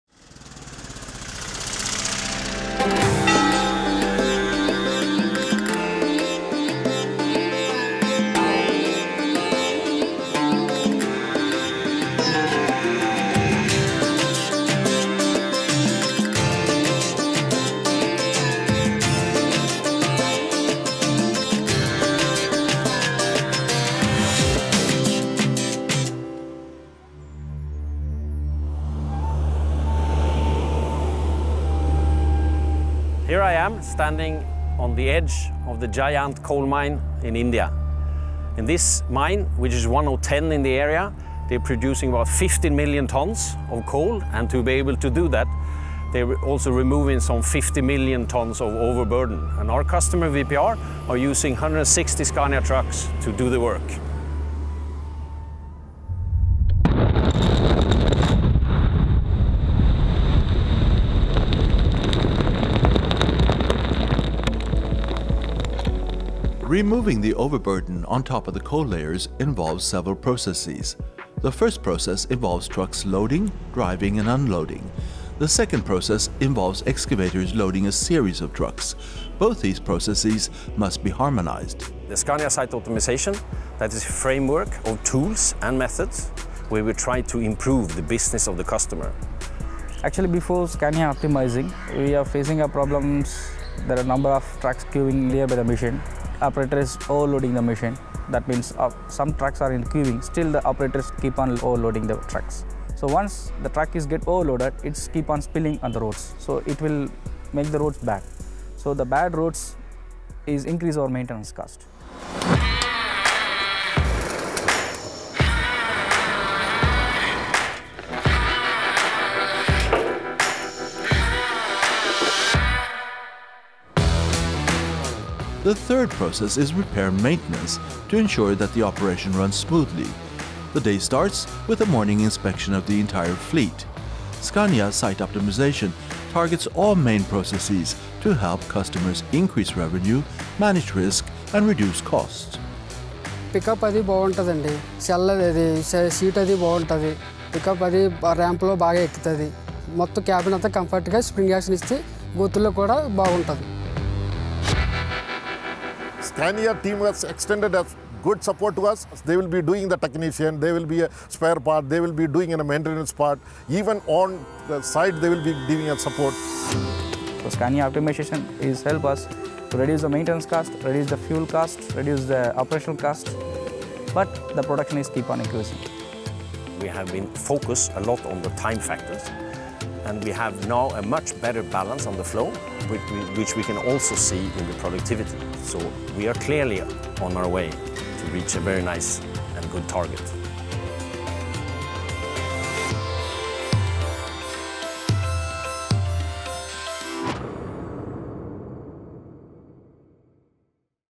Scania Site Optimisation in India
Underneath the hot sun and haze, a team from Scania Mining and the customer, VPR Mining Infrastructure Pvt Ltd., are working with an ongoing Scania Site Optimisation Project – a framework of tools, methods, and information to improve mining transport operations – at the coal mine Jayant in Singrauli in the Indian state of Madhya Predesh.